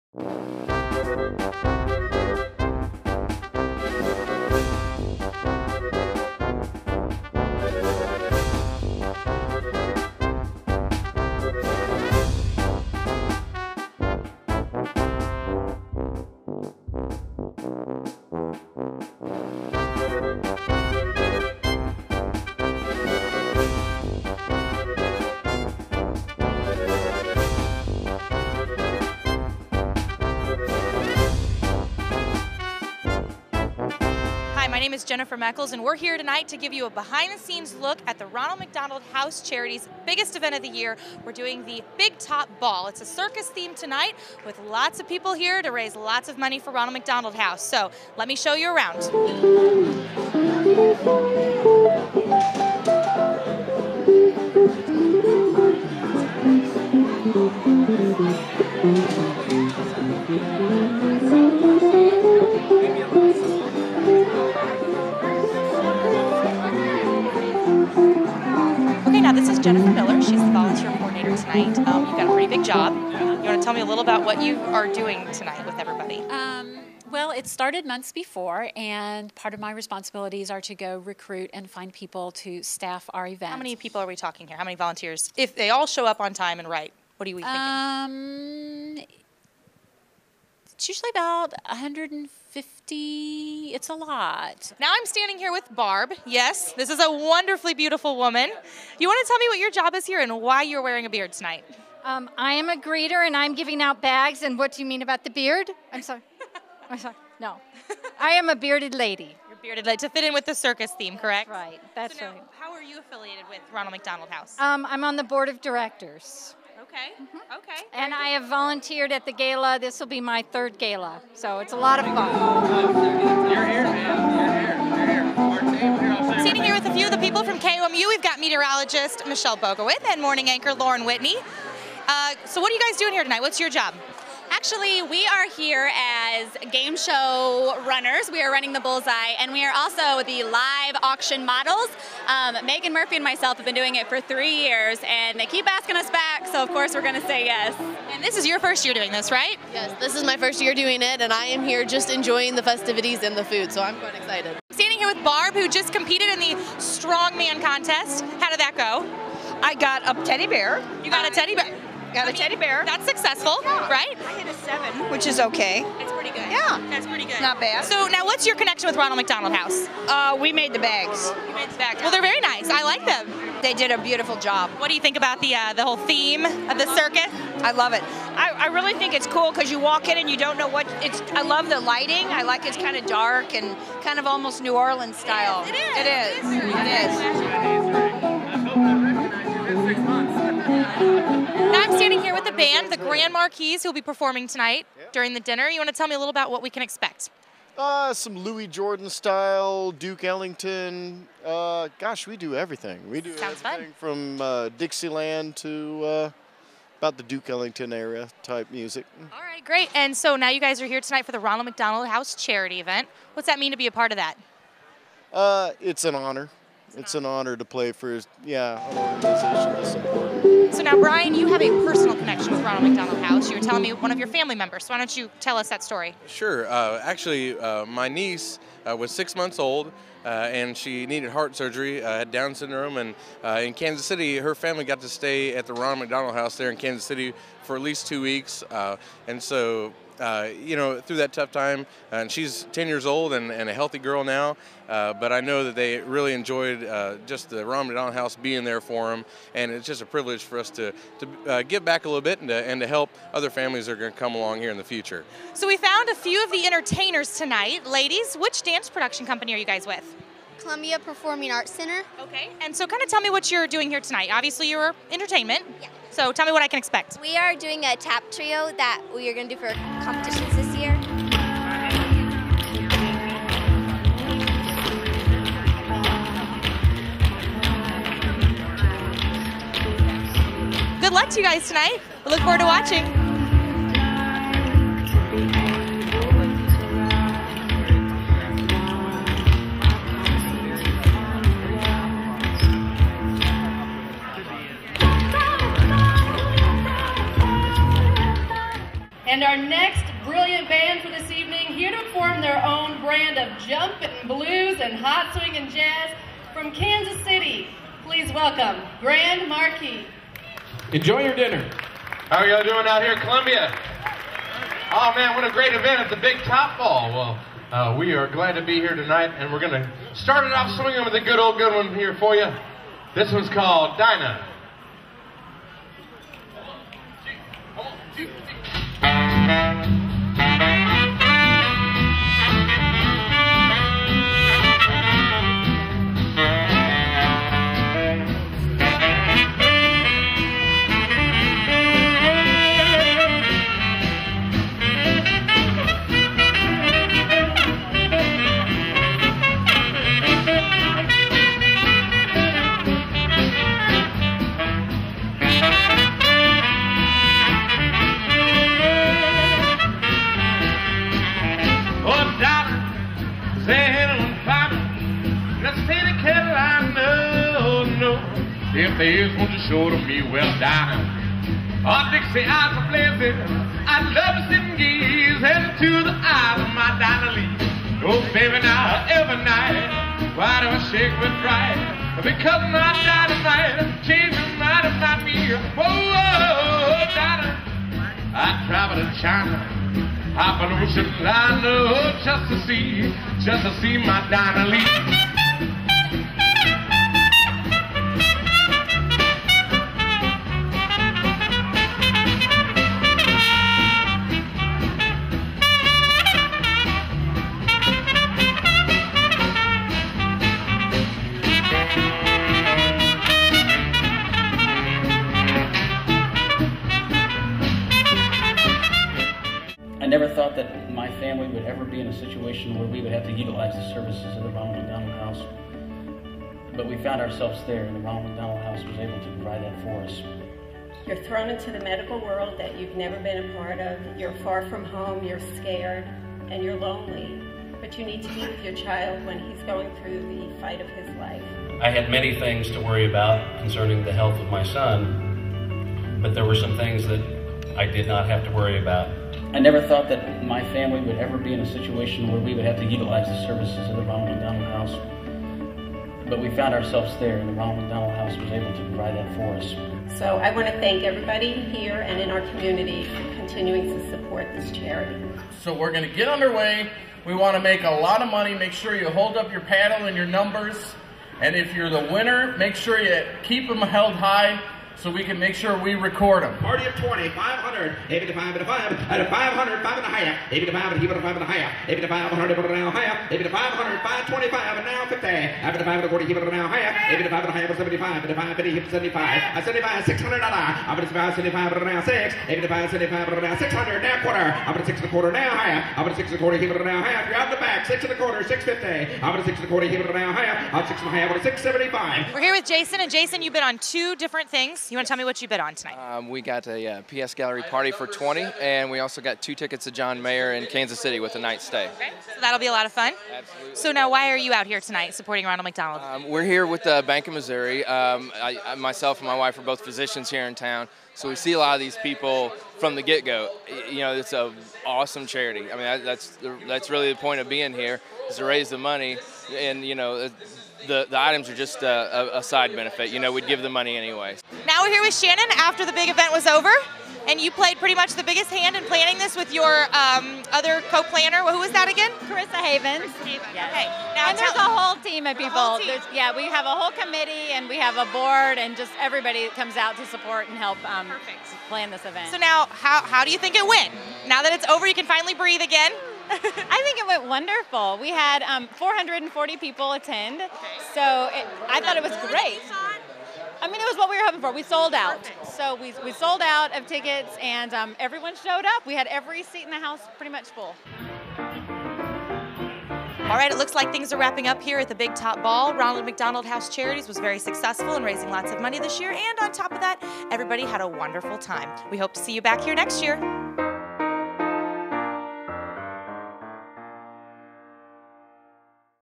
The Big Top Ball 2010 - Ronald McDonald House Charities Columbia, MO
This circus themed gala in Columbia, MO is the biggest fundraiser of the year for the local Ronald McDonald House.